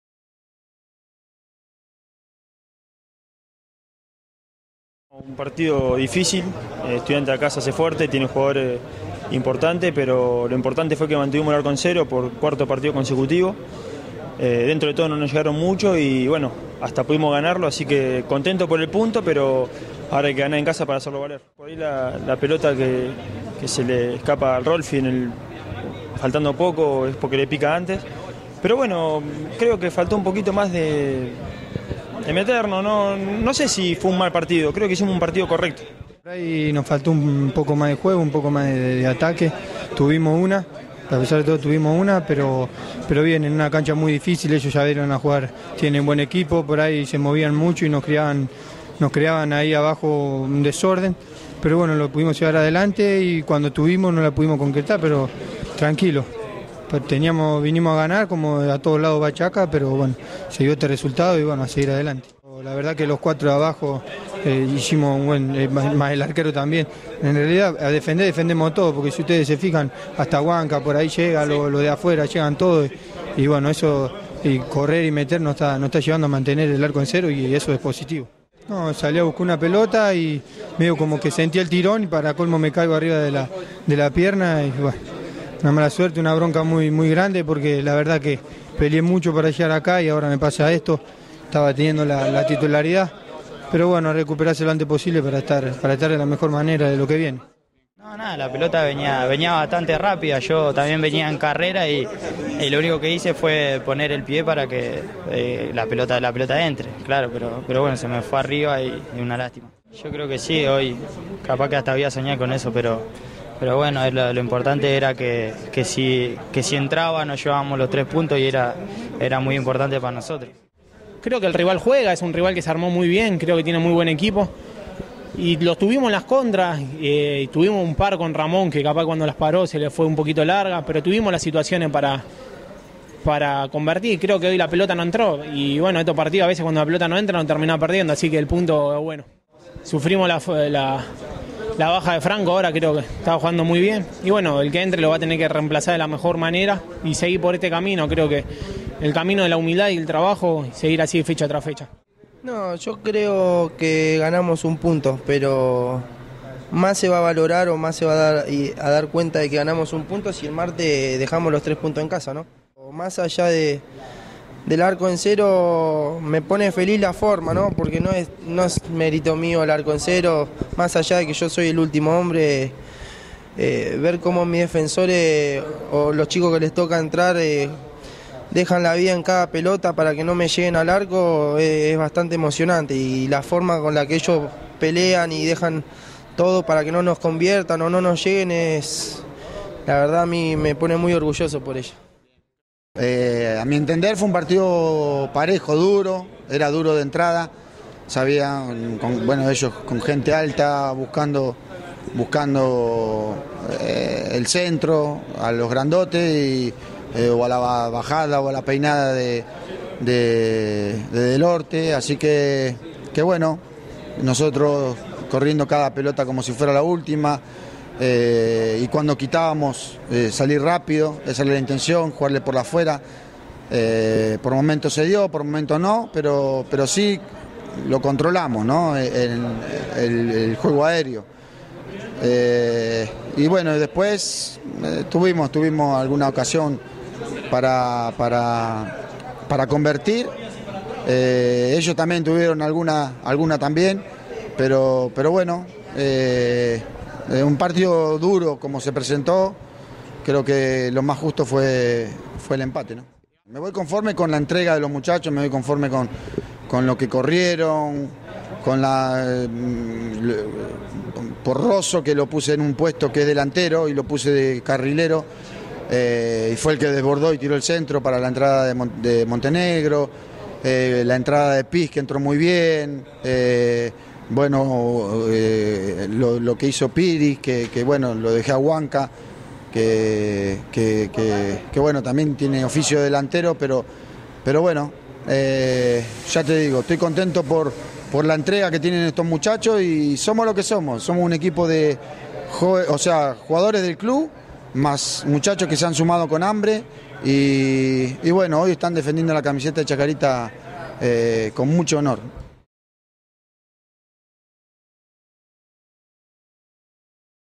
ChacaTV -Notas post Estudiantes BsAs 0 Chacarita 0
Notas post Estudiantes BsAs 0 Chacarita 0
FACUNDO LAUMANN
FRANCO RACCA
MAURO MONTENEGRO
GONZALO ROCANIERE
FERNANDO OTAROLA
CARLOS LEEB
Producido por el Departamento de Prensa del Club Atlético Chacarita Juniors